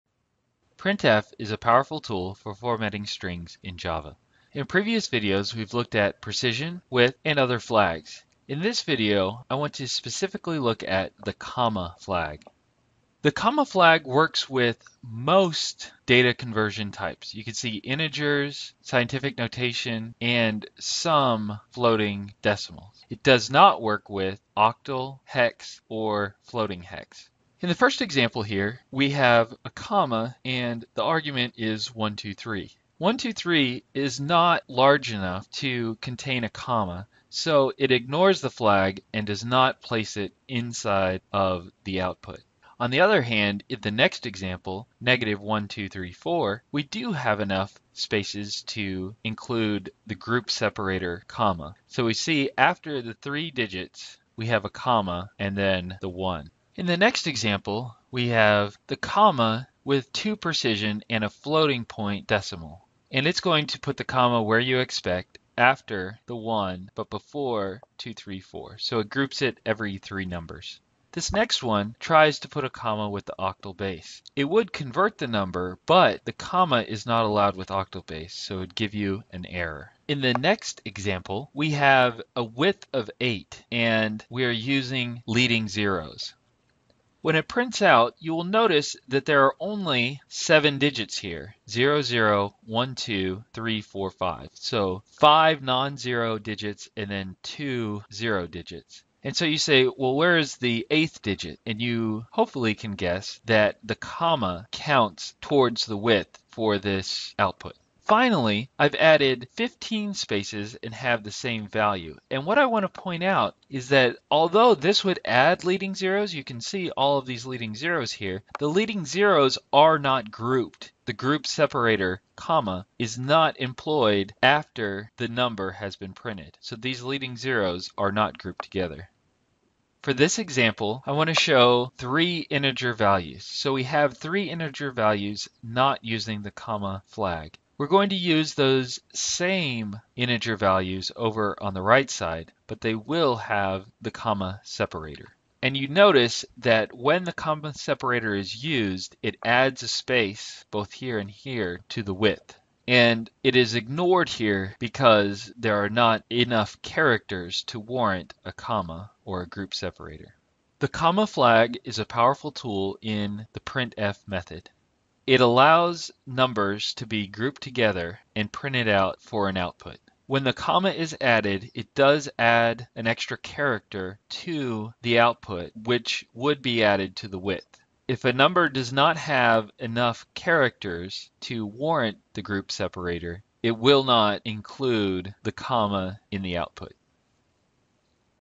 Print Formatting Part 9: printf() Flag , (Java)
This video looks at the flag , and how it affects the printf() method.